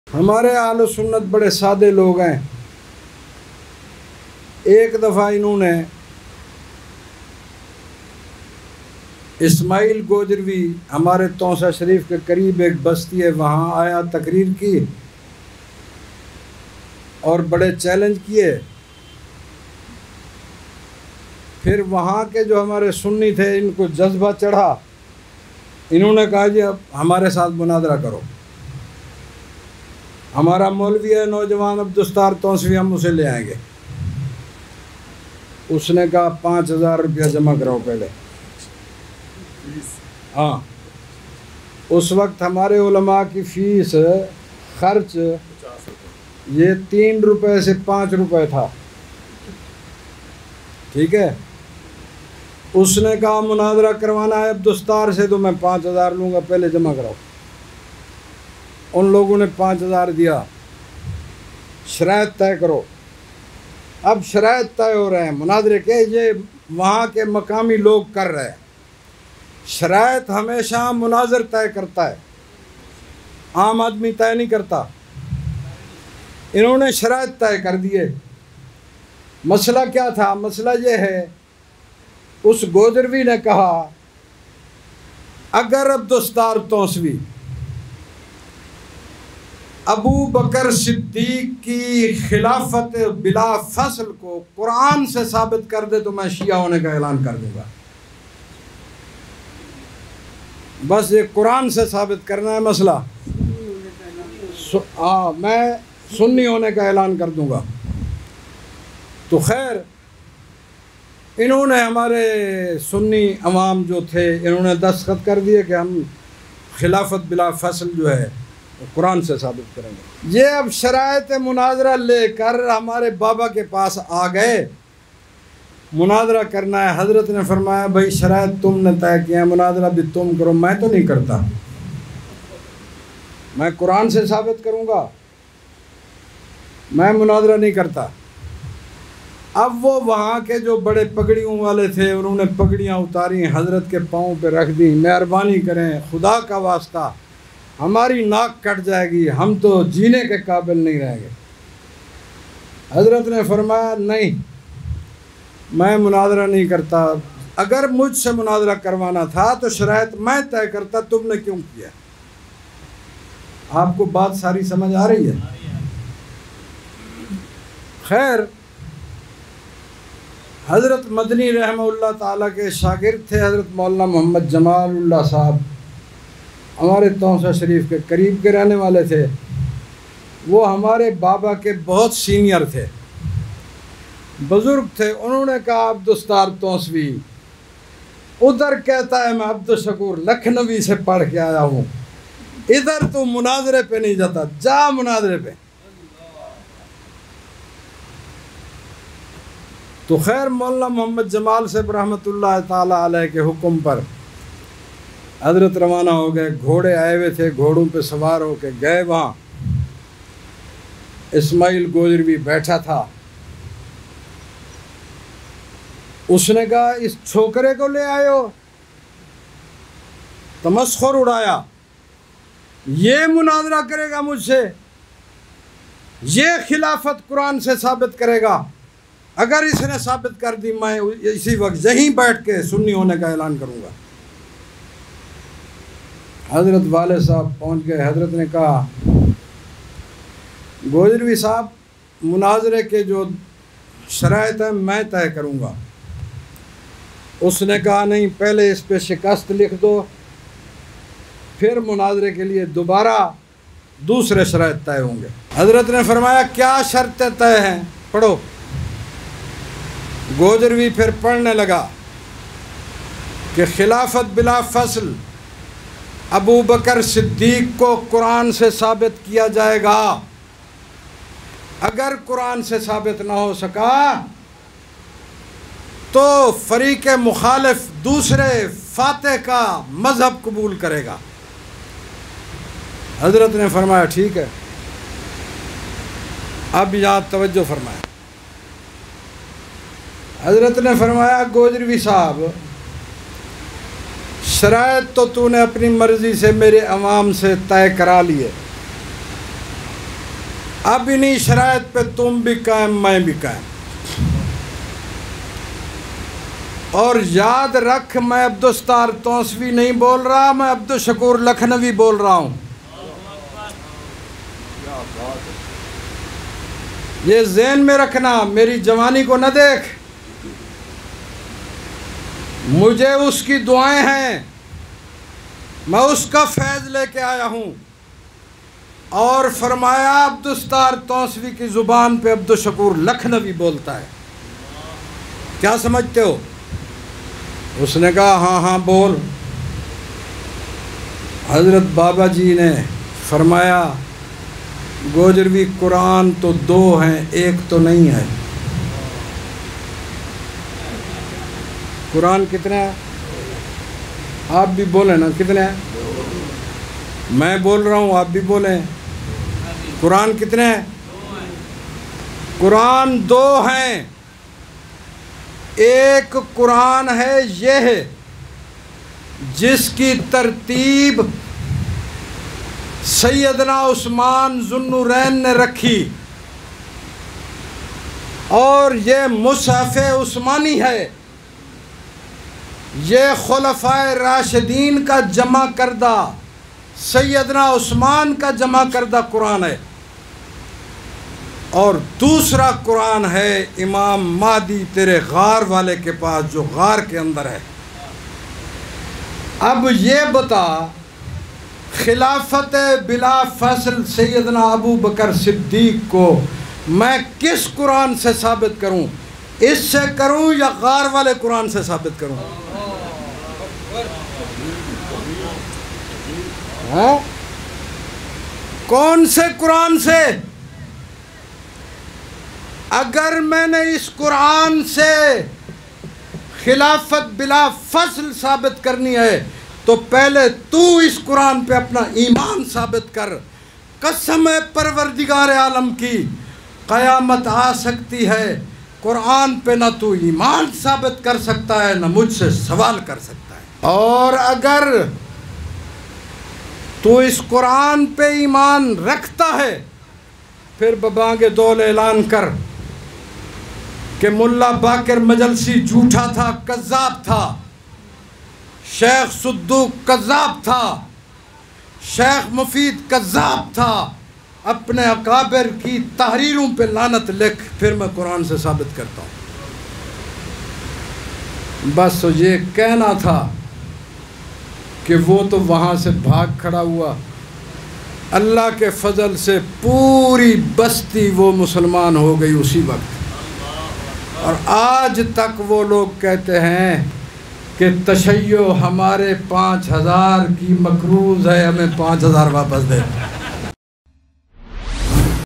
Maulana Abdul Sattar Taunsvi ka Shia se MUNAZARA ! Shia Challenge & Reply شیعہ سے مناظرہ
حضرت ابوبکر صدیق رضی اللہ تعالی عنہ کو قرآن سے خلیفہ بلا فصل ثابت کر دو میں شیعہ مذہب چھوڑ دوں گا شیعہ کا چیلنج
مولانا عبدالستار تونسویؓ کا شیعہ سے مناظرے کی دلچسپ کارگزاری